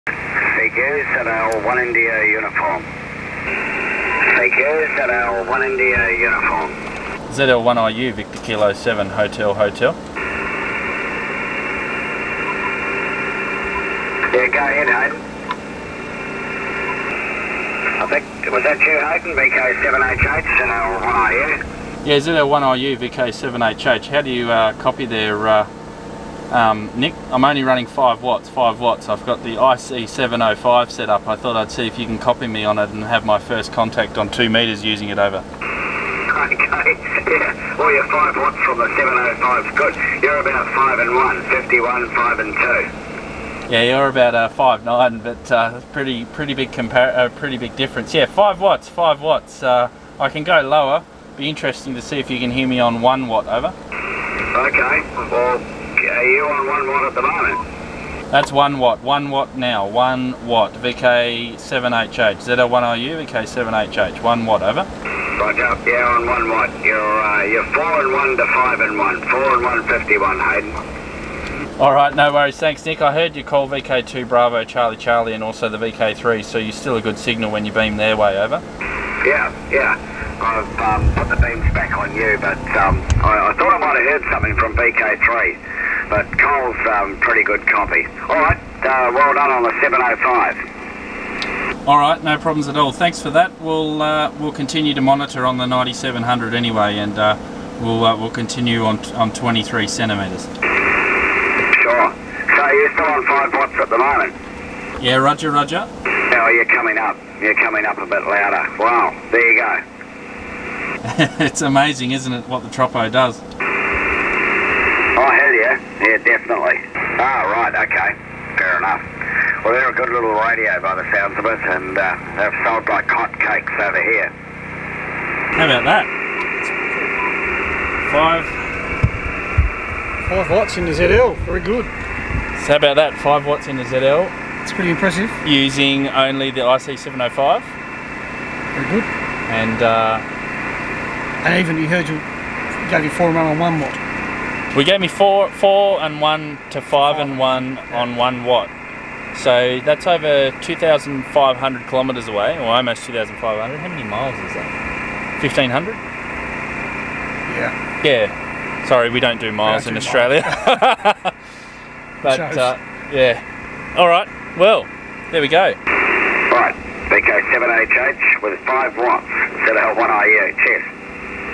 We Worked into ZL with 1 Watt using an IC-705 on 144 MHz!!
Yes that's right, a 1 watt contact using a Icom IC-705 on the 144 MHz (2 meter) band across to New Zealand (ZL).

Strong tropospheric propagation was responsible for such an event happening. We also worked New Zealand on other higher bands too! Check out the video here:

If you enjoyed this video help others enjoy it by adding captions in your language: